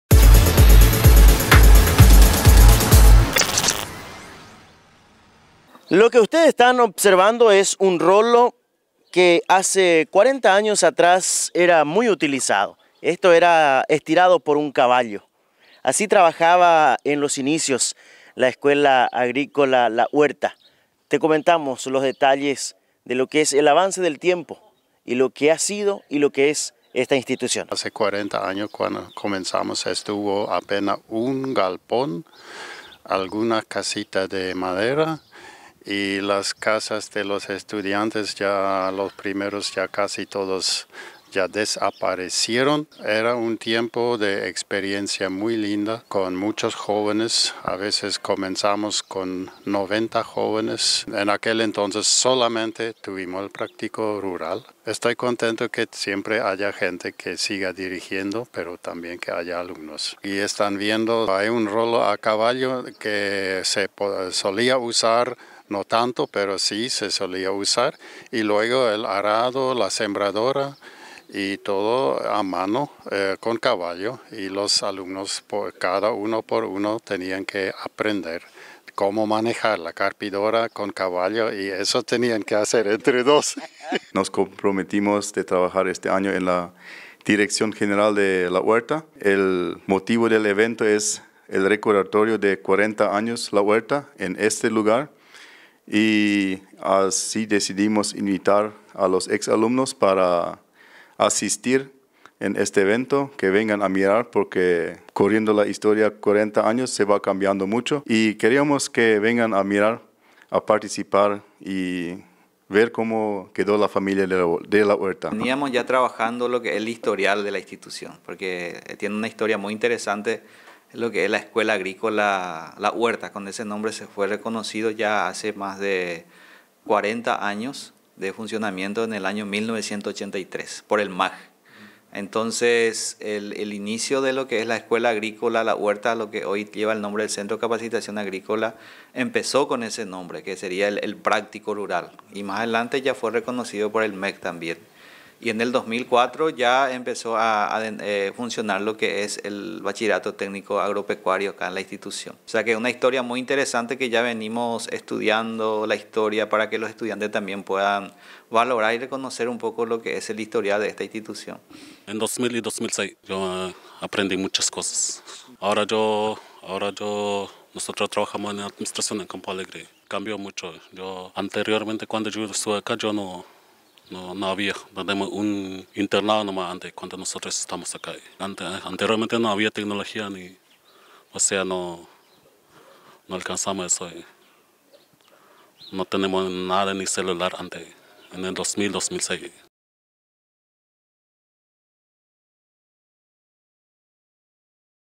El Centro de Capacitación Agrícola la Huerta cumple 40 años de fundación y reunió a ex alumnos
Radio ZP30 – La Voz del Chaco Paraguayo
Estudio Central, Filadelfia, Dep. Boquerón
Contáctenos
Síganos
  / radiozp30
  / zp30.espanol